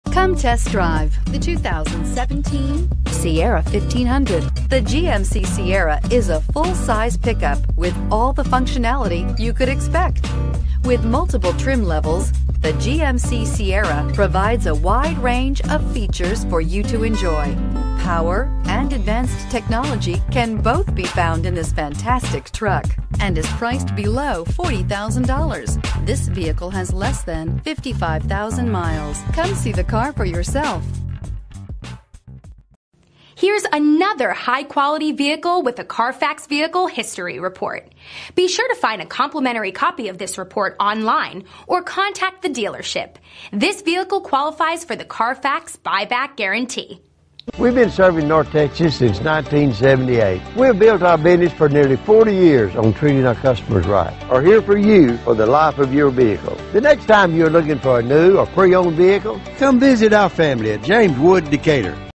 2017 GMC Sierra 1500 Decatur TX 210963A1
2017 GMC Sierra 1500 SLT

2111 US HWY 287 South

Pick up this Cardinal Red 2017 GMC Sierra 1500, available today at James Wood Motors. This could be the one you've been searching for.